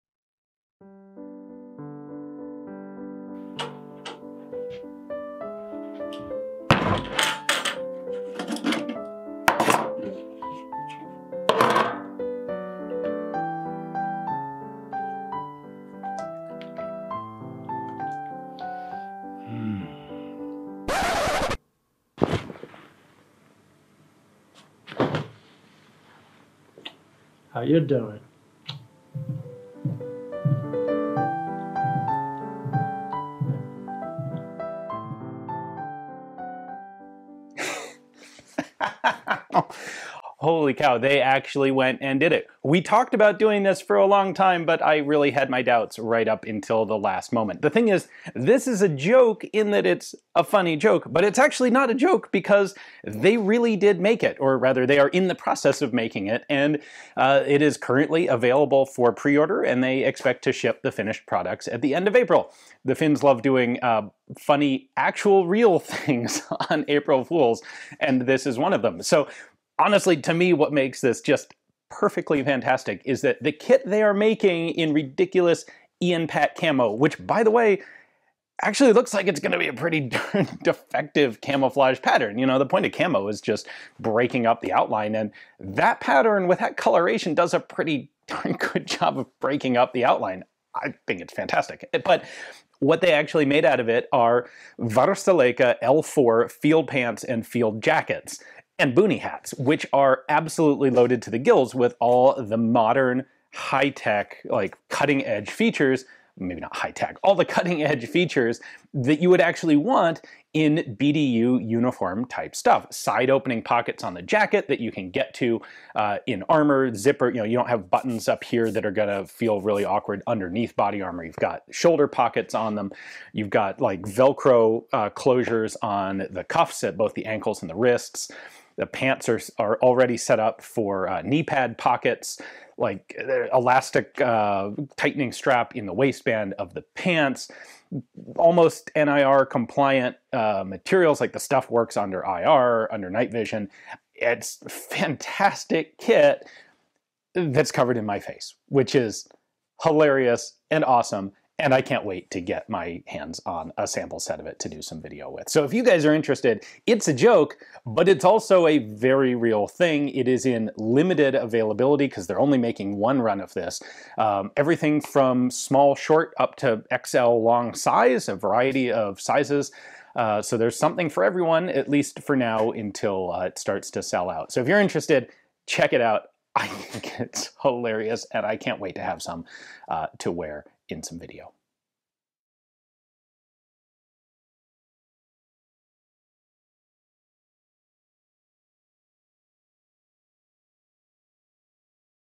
THEY ACTUALLY DID IT! And it's not an April Fool's Joke....
We talked about making actual IanPat camo, but I wasn't really sure that Varusteleka would have the hutzpah to actually do it...but they did! And it's actually real! They made a whole order of custom IanPat camo material and made it into L4 Field Jackets, L4 Combat Pants, and Boonie Hats. With all the same excellent features of their regular kit, these are hilarious...and actually really good kit. Heck, it even works well as camouflage!